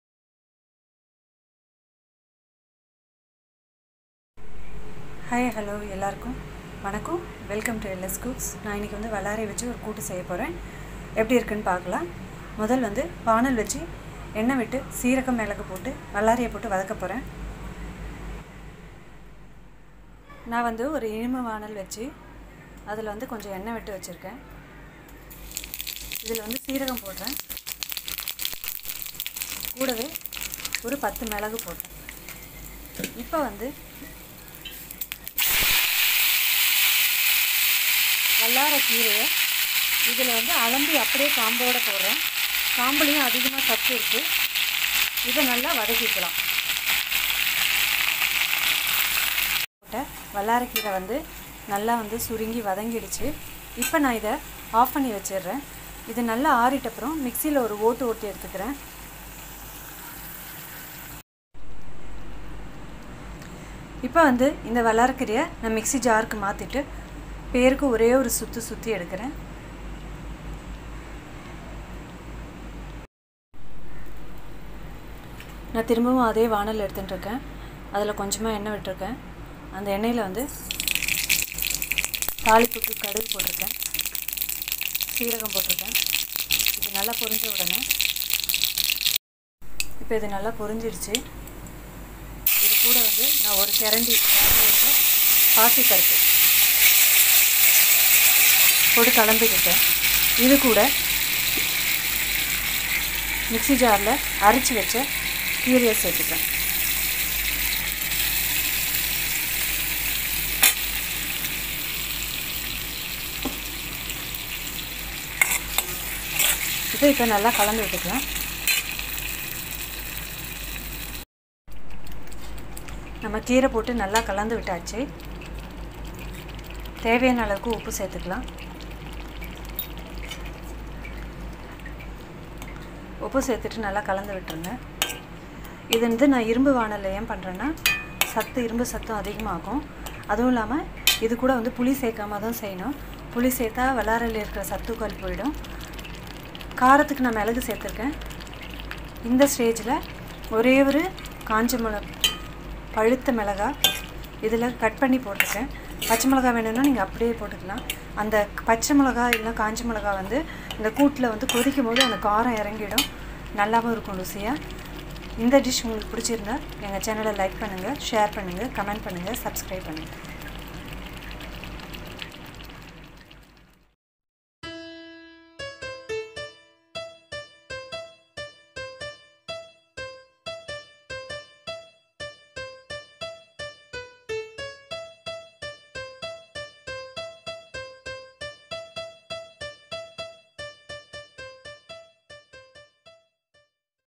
வல்லாரைக்கீரையை இப்படி செஞ்சு பாருங்கள் / vallarai keerai recipe
vallarai keerai
moong dal
mustard seeds
cuminseed
pepper
salt
greenchilli